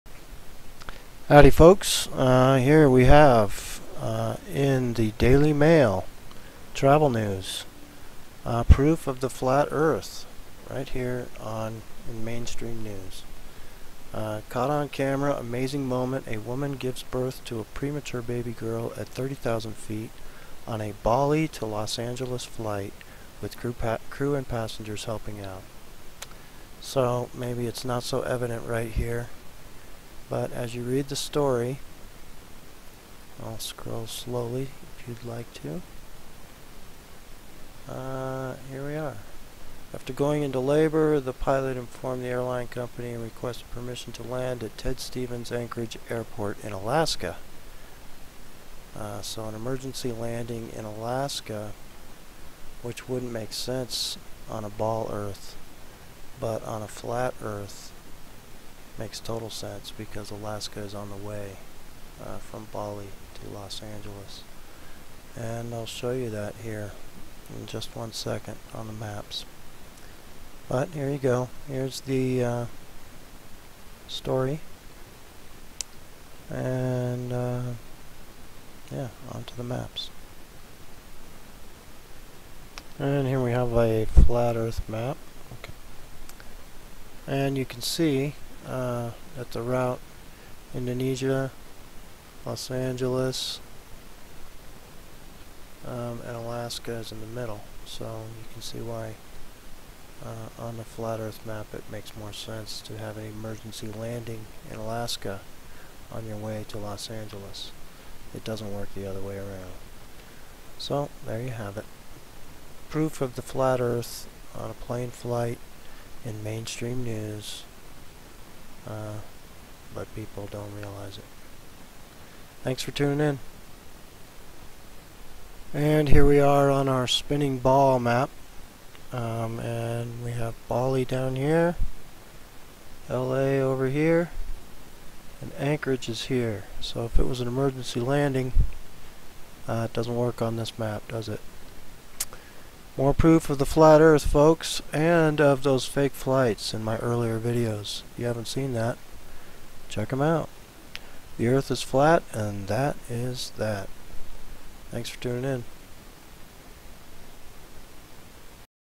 Flat Earth | Emergency Birth Proves The Earth Is FLAT - ( Awesome Proves )
A woman gives birth on the flight from Bali to Los Angeles and proves the Earth is flat by an emergency landing in Alaska. After going into labor, the pilot informed the airline company and requested permission to land at Ted Stevens Anchorage Airport in Alaska, which is impossible on a spinning ball but makes perfect sense on a flat Earth.